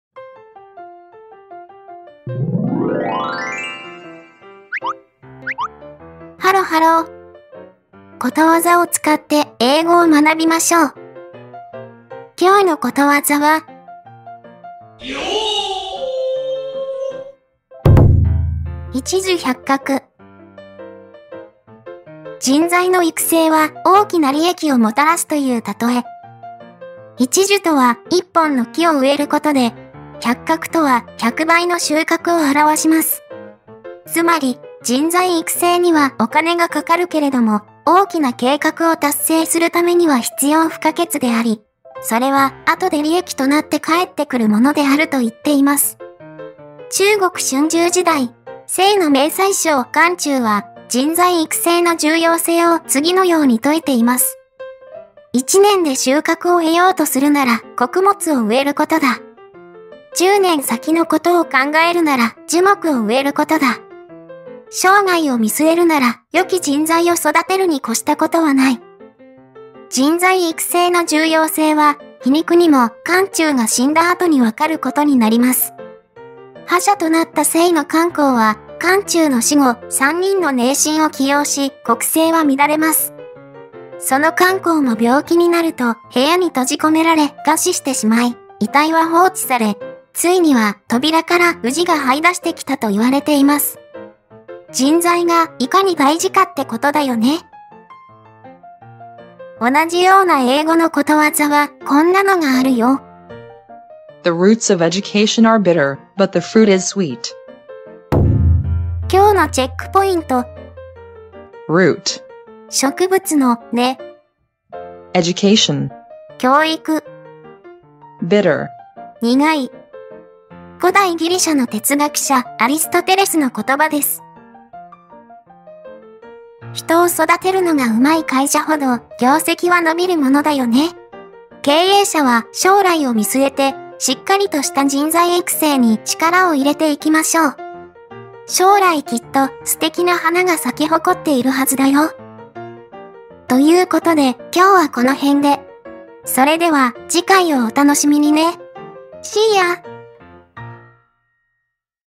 【一樹百穫】ことわざを使って英語を学ぼう！
ことわざを使って英語を学びましょう。

【諺】一樹百穫
人材の育成は大きな利益をもたらすという喩え。
出典：『管子 - 権脩』

一樹とは、一本の木を植えることで、百穫とは、百倍の収穫を表します。
人材育成にはお金がかかるけれども、大きな計画を達成するためには必要不可欠であり、それはあとで利益となって返ってくるものであると説いています。

【英】The roots of education are bitter, but the fruit is sweet.
『教育の根は苦いが、その果実は甘い』
・root：〔植物の〕根
・education：教育
・bitter：苦い

▸古代ギリシャの哲学者のアリストテレスの言葉です